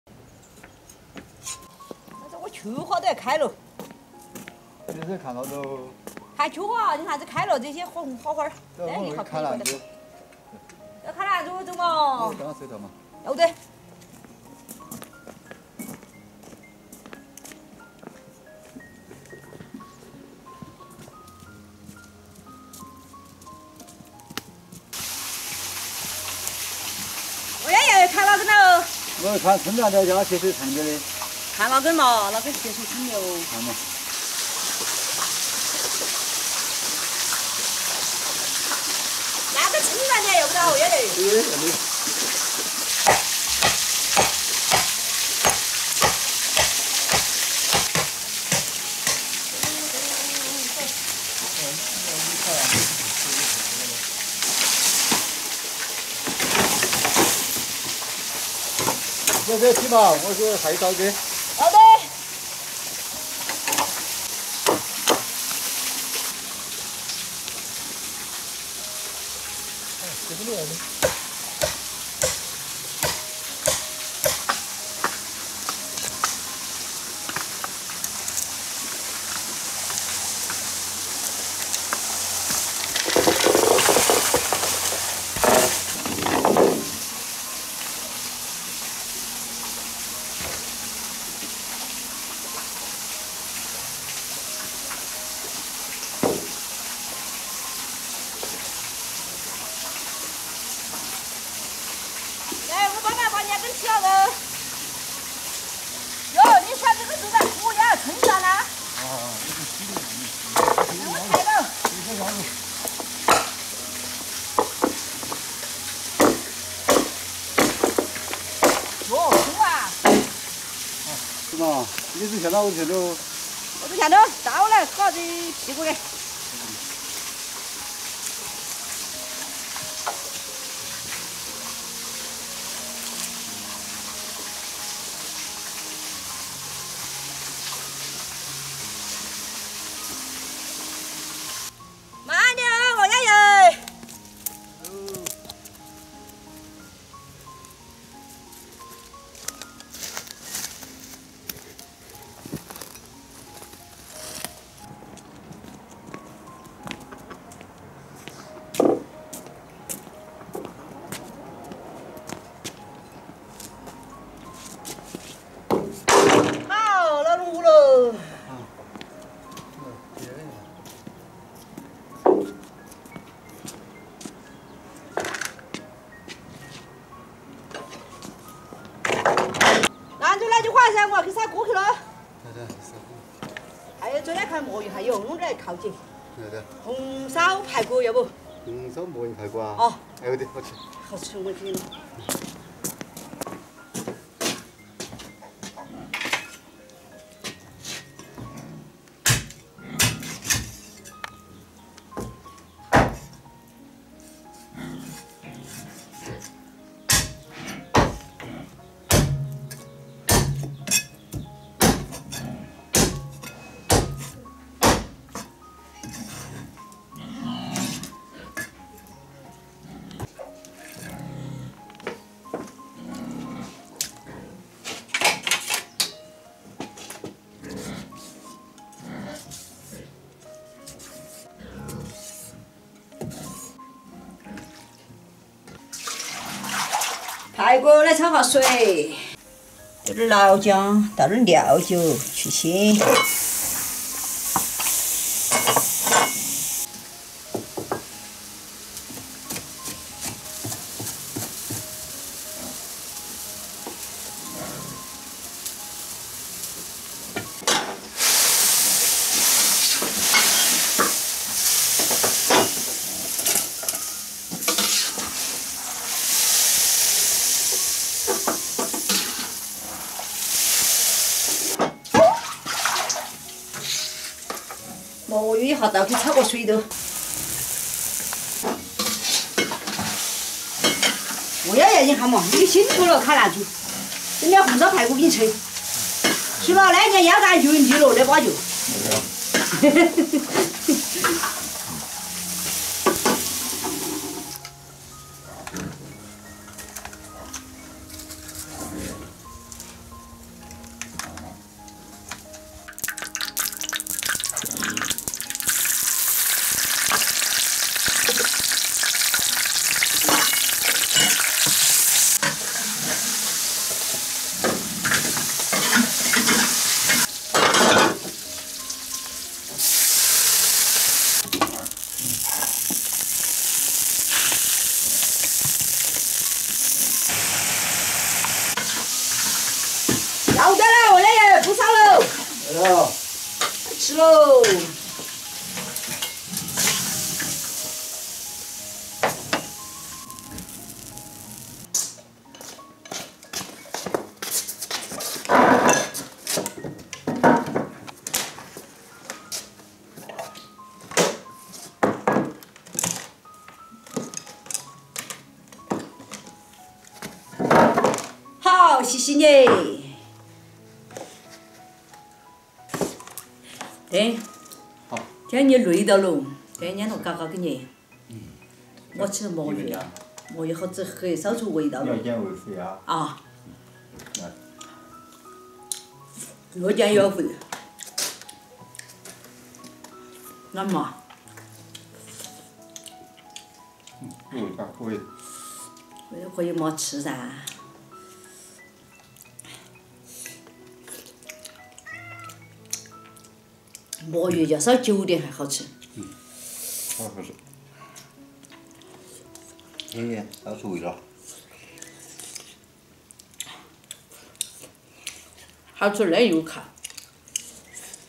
【ENG SUB】王幺爷砍楠竹太卖力，幺妈做啥犒劳？一盘魔芋烧排骨满足了【幺妈和幺叔】
【请放心，本视频是一部原创拍摄的短剧，没有任何不良行为。】
王幺爷喊幺妈上山砍楠竹，仔细挑选一番后选定两根竹子，夫妻俩一前一后背回家，做道排骨犒劳自己，安逸！